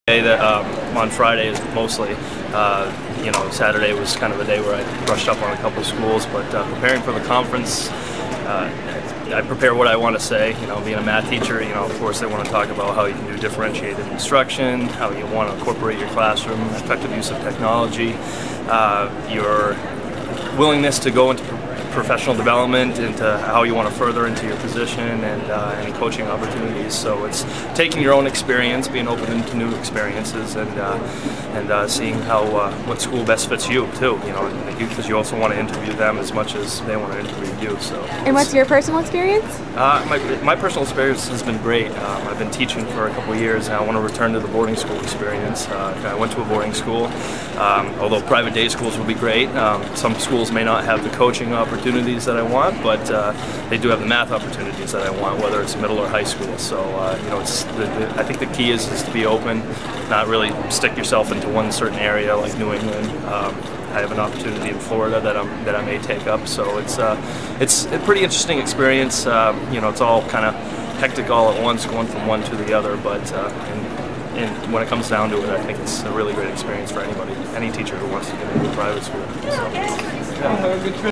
Conference Experience: Math Teacher/Hockey Coach
This triple-threat (teacher, coach, and dorm parent) shares his experience and advice for attending a Carney, Sandoe  & Associates teacher hiring conference.